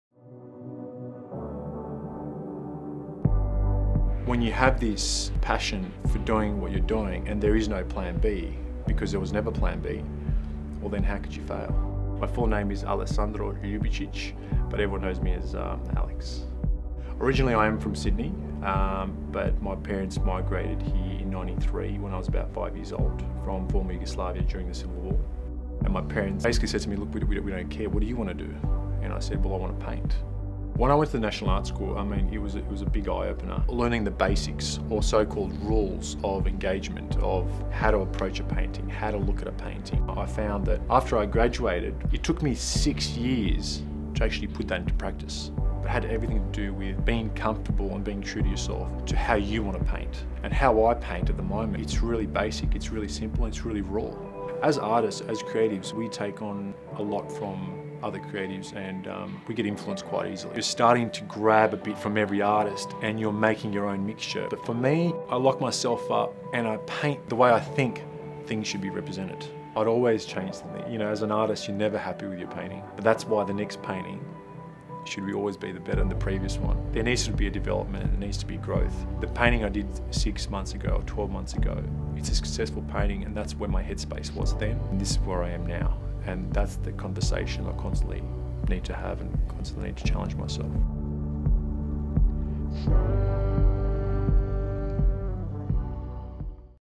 The Studio Of Alesandro Ljubicic
Alesandro Ljbuicic in his Sydney studio, October 2016.